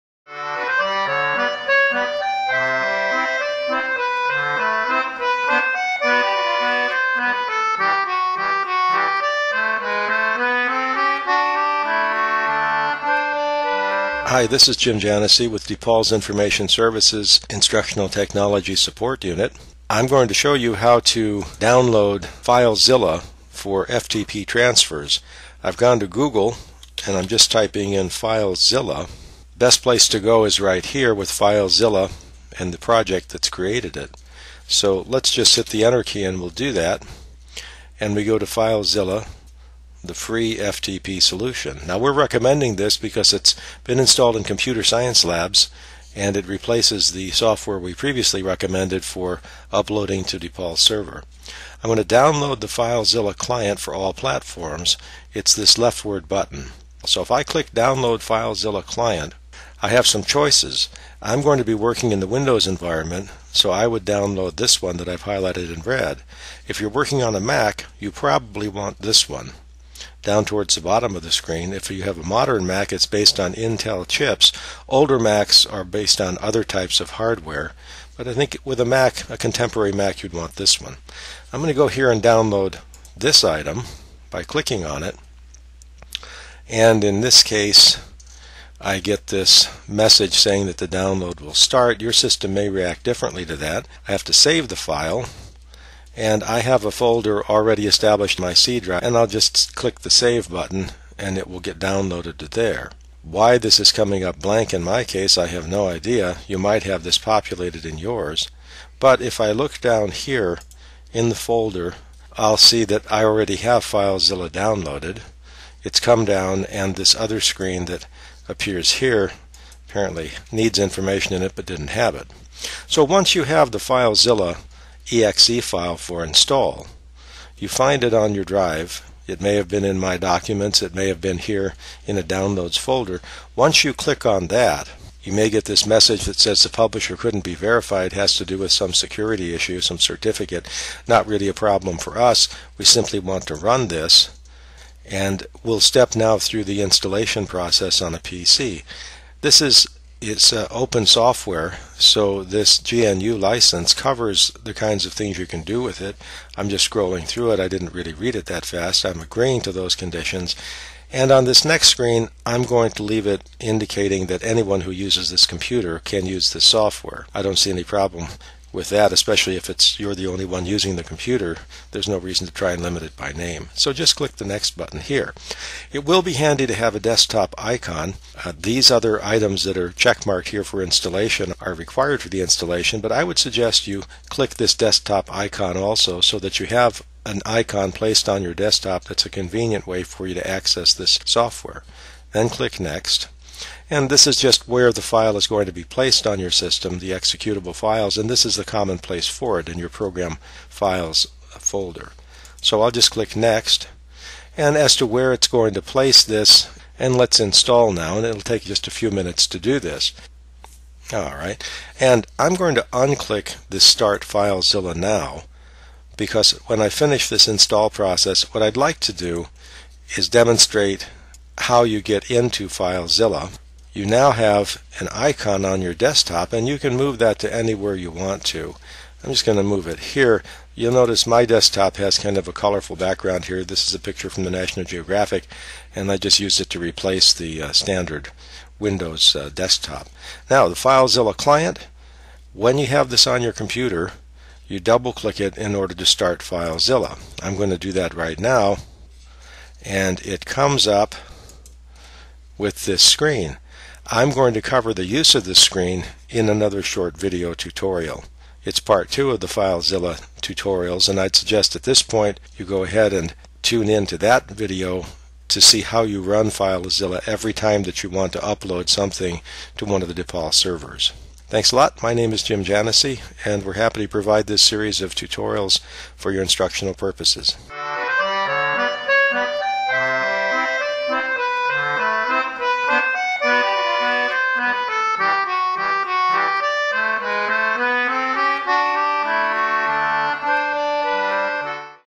Filezilla Part 1: Download and Install
Part 1 of a 2-part video tutorial on FileZilla, a free FTP utility program for uploading files to web servers. Versions are available for Macs as well as PCs.